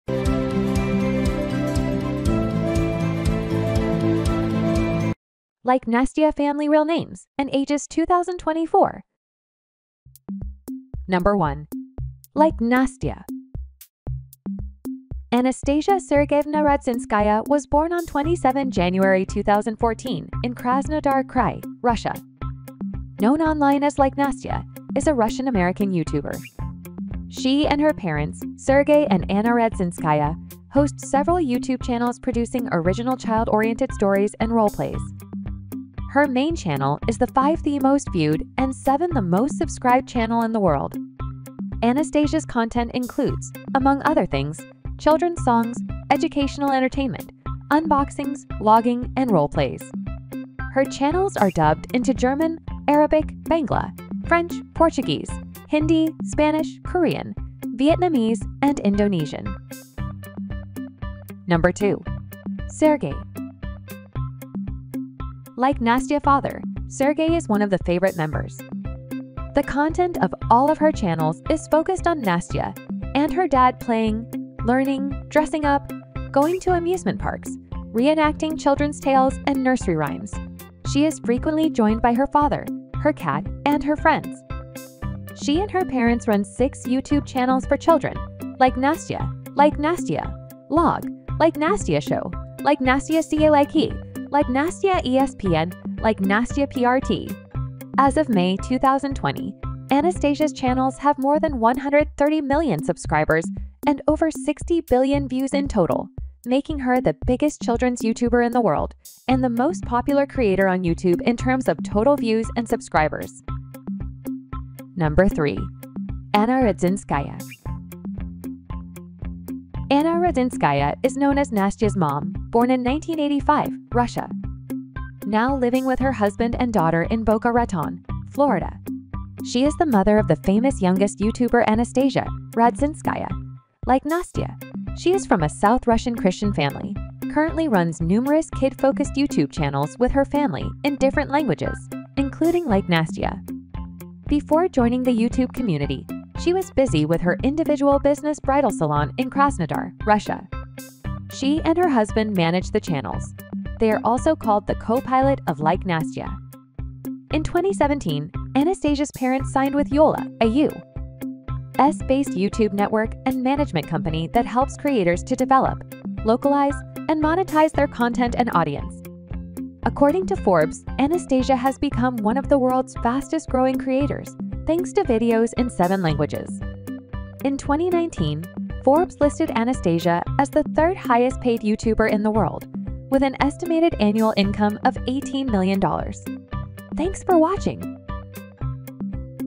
Like Nastya Family Real Names & Ages 2024
1. Like Nastya
Anastasia Sergeyevna Radzinskaya was born on 27 January 2014 in Krasnodar Krai, Russia. Known online as Like Nastya, is a Russian-American YouTuber. She and her parents, Sergey and Anna Radzinskaya, host several YouTube channels producing original child-oriented stories and roleplays. Anastasia's content includes, among other things, children's songs, educational entertainment, unboxings, vlogging, and roleplays. Her channels are dubbed into German, Arabic, Bangla, French, Portuguese, Hindi, Spanish, Korean, Vietnamese, and Indonesian.

 2. Sergey
Like Nastya father Sergey is one of the favorite members. The content of all of her channels is focused on Nastya and her dad playing, learning, dressing up, going to amusement parks, reenacting children's tales and nursery rhymes. She is frequently joined by her father, her cat, and her friends. She and her parents run six YouTube channels for children, Like Nastya, Like Nastya Vlog, Like Nastya Show, Like Nastya AE, Like Nastya ESP, and Like Nastya PRT. As of May 2020, Anastasia's channels have more than 130 million subscribers and over 60 billion views in total, making her the biggest children's YouTuber in the world and the most popular creator on YouTube in terms of total views and subscribers.

3. Anna Radzinskaya
Anna Radzinskaya is known as Nastya's Mom, born in 1985, Russia. Now living with her husband and daughter in Boca Raton, Florida. She is the mother of the famous youngest Youtuber Anastasia Radzinskaya (Like Nastya). She is from a South Russian Christian Family, Currently runs numerous kid-focused YouTube channels with her family in different languages, including Like Nastya. Before joining the YouTube community, she was busy with her individual business bridal salon in Krasnodar, Russia. She and her husband manage the Channels. They are also called the Co-pilot of Like Nastya. In 2017, Anastasia's parents signed with Yoola, a U.S-based YouTube network and management company that helps creators to develop, localize, and monetize their content and audience. According to Forbes, Anastasia "has become one of the world's fastest-growing creators, thanks to videos in seven languages." In 2019, Forbes listed Anastasia as the third highest-paid YouTuber in the world, with an estimated annual income of $18 million.